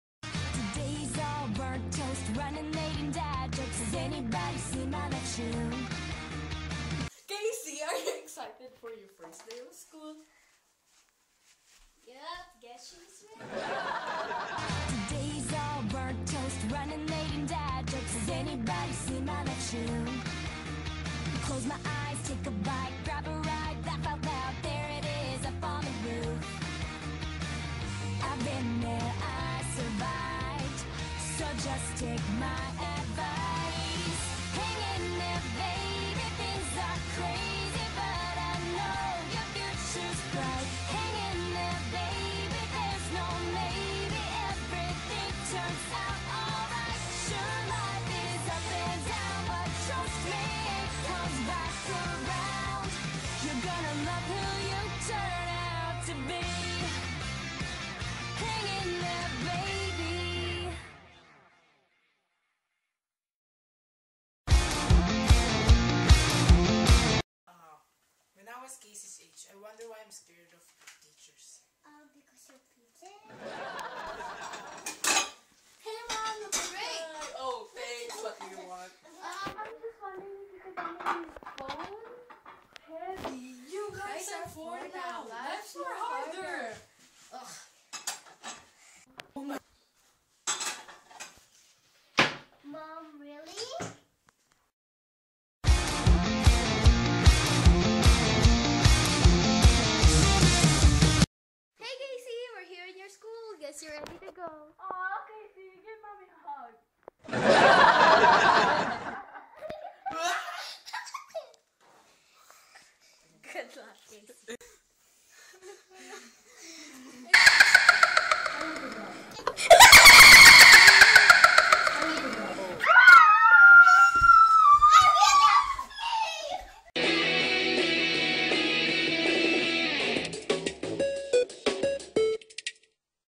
Good luck KC (Season1 episode 1) Kc's first day of school.
The series follows two teenagers, Teddy Duncan (Jenelle Delarosa) and Paul James (P.J.) Duncan (Ervin Delarosa), and  their 10-year-old sister, Gabriella Duncan (Frances Aranzaso) . whose parents (Amy Duncan- Roshayne Aranzaso) enlist their help when they return to work after the birth of their fourth child, KC. With their parents juggling full time careers, the older Holliday siblings pitch in, getting very familiar with baby formula, burps, clothes, and babysitting while navigating typical teenage life. In each episode, Teddy creates a personalized video diary for KC. Each video will chronicle Teddy's daily adventures and mishaps for KC to use as advice when her big sister is no longer living at home.